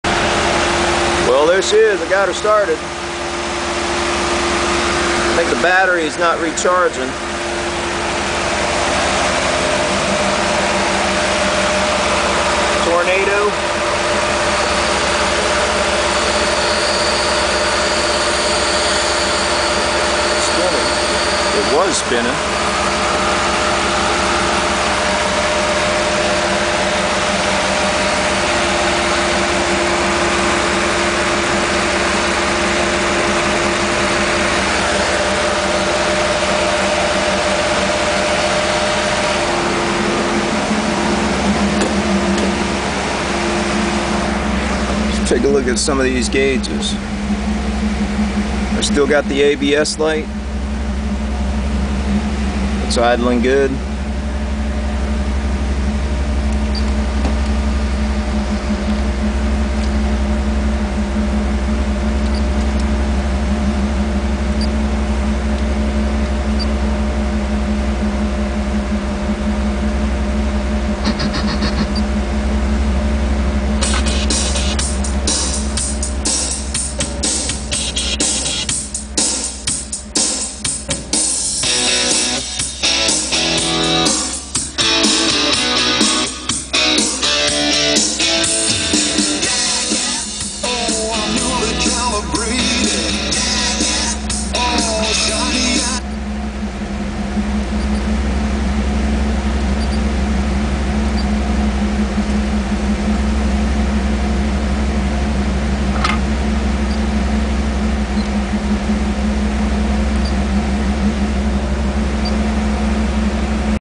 First Start After Major Overhaul - 1994 Pontiac Trans Am GT Convertible
After just about everything from transmission, radiator, rewiring battery, power steering hose, rewiring rear lights, convertible top, headlight motor, window regulator, front and rear brake work, all fluid flush, transmission flush and filter, and a few more things; she starts up for the first time.